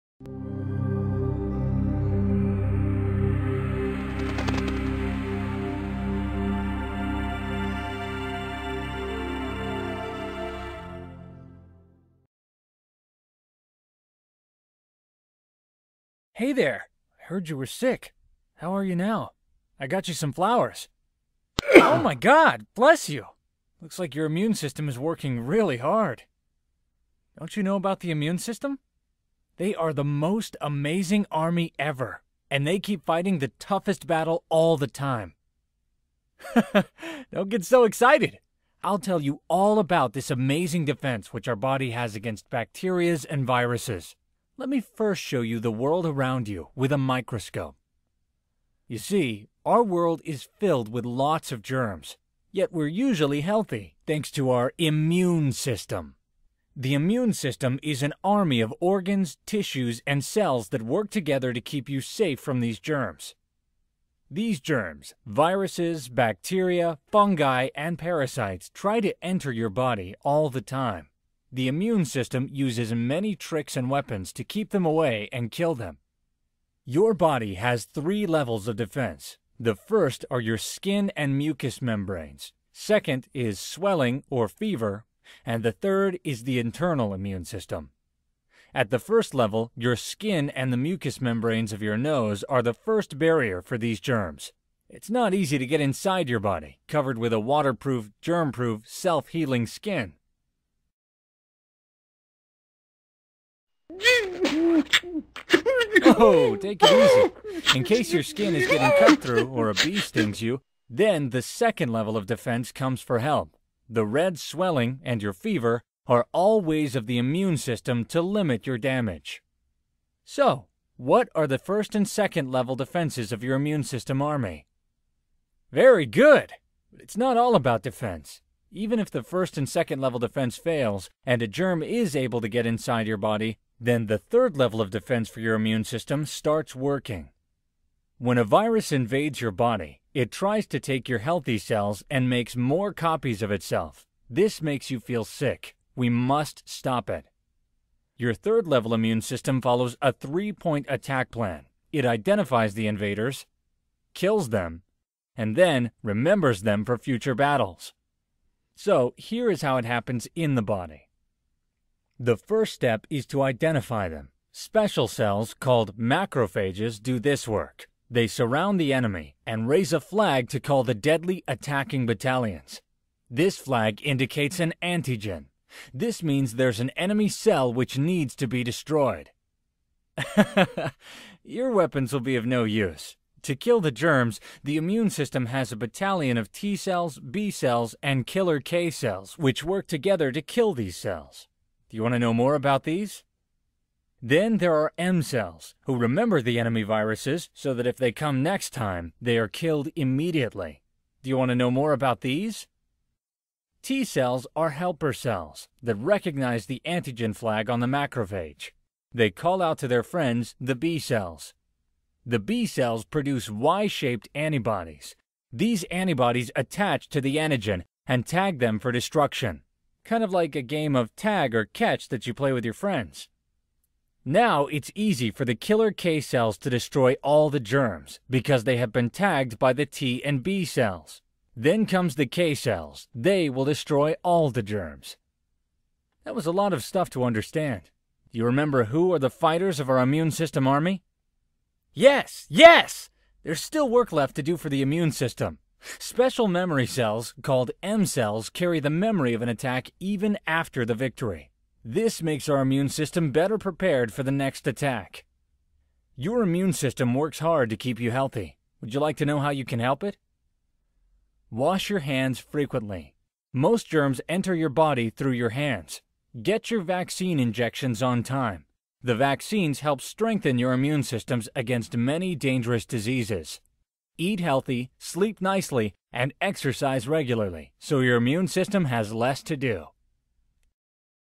Human Immune System for kids - Body Defense Mechanism ( Biology )
for more free science videos.

The Immunological System parts:  Recognition, Attack, and Memory.